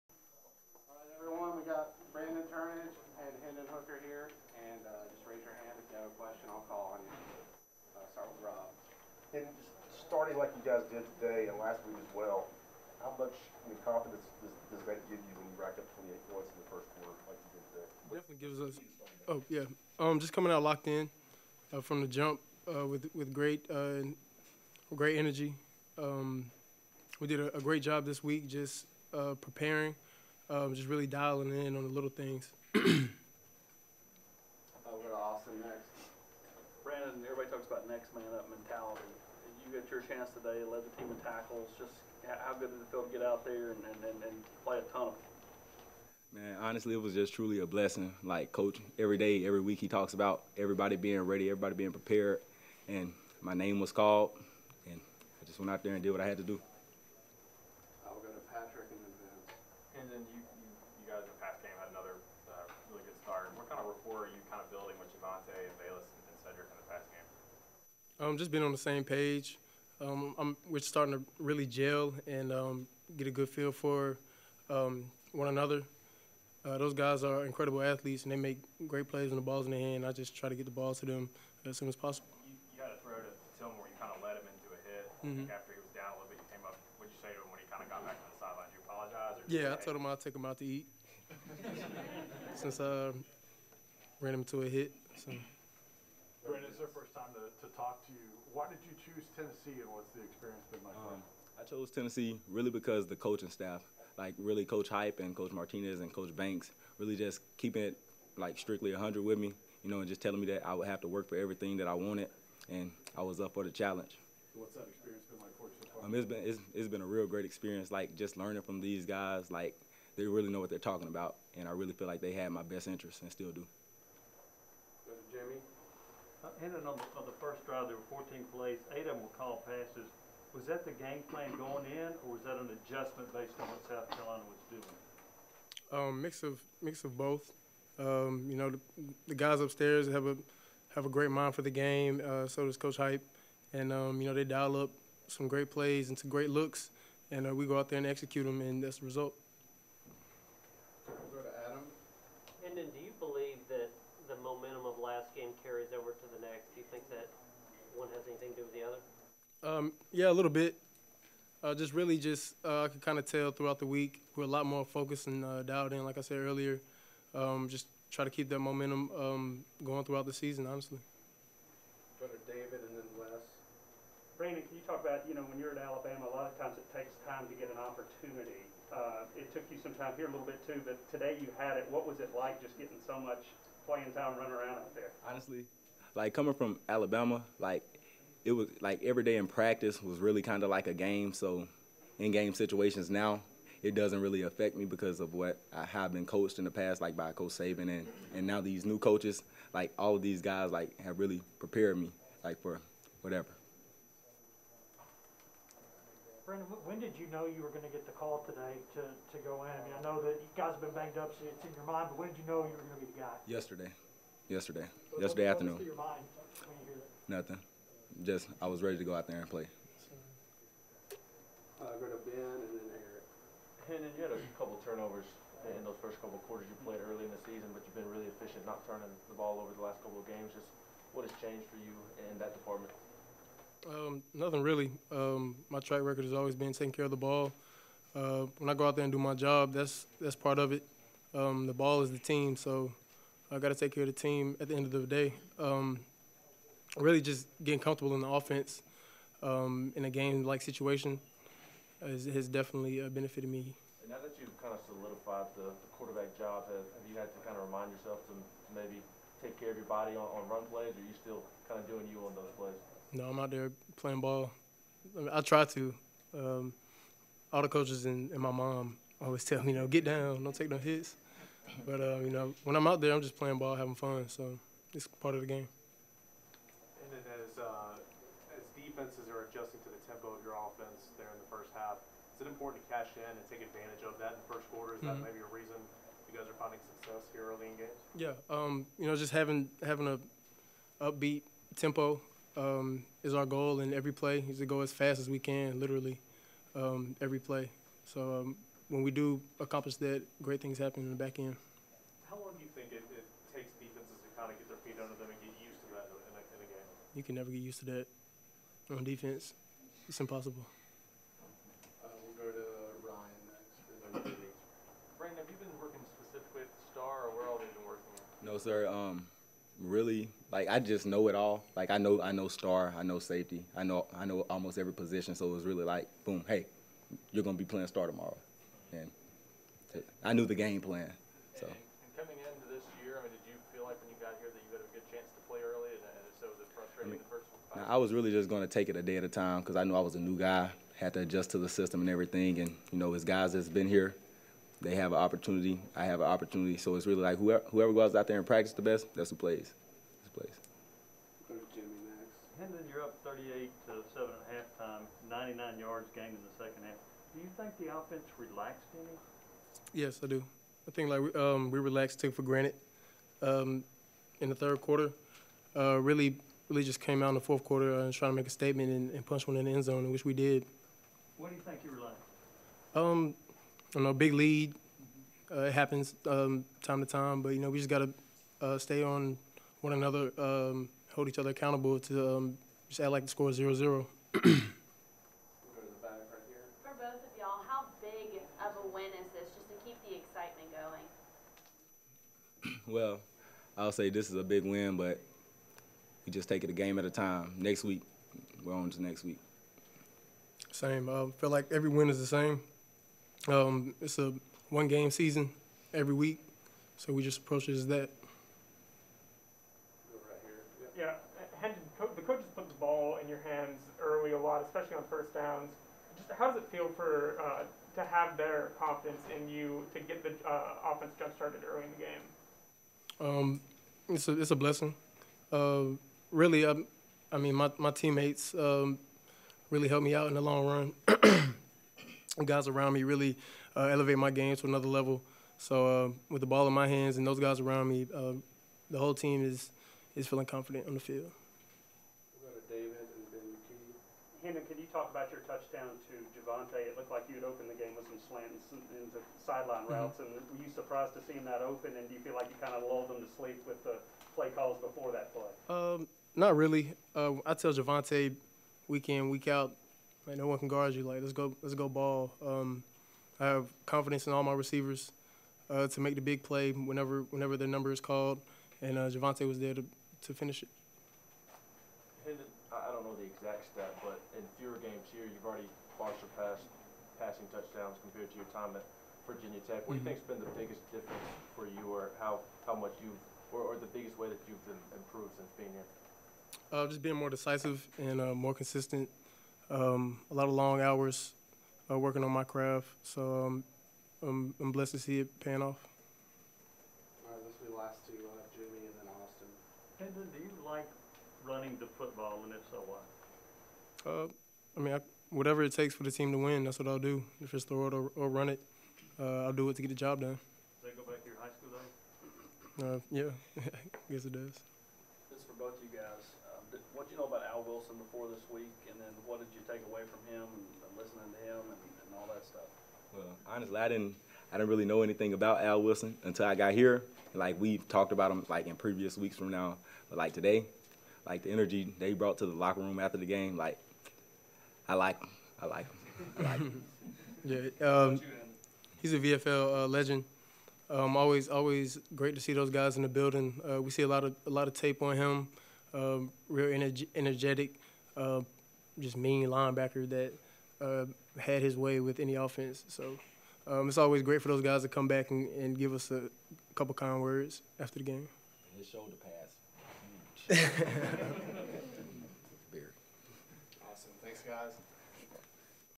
Hendon Hooker Brandon Turnage South Carolina win postgame Tennessee Volunteers Oct. 9, 2021
Video from Tennessee Athletics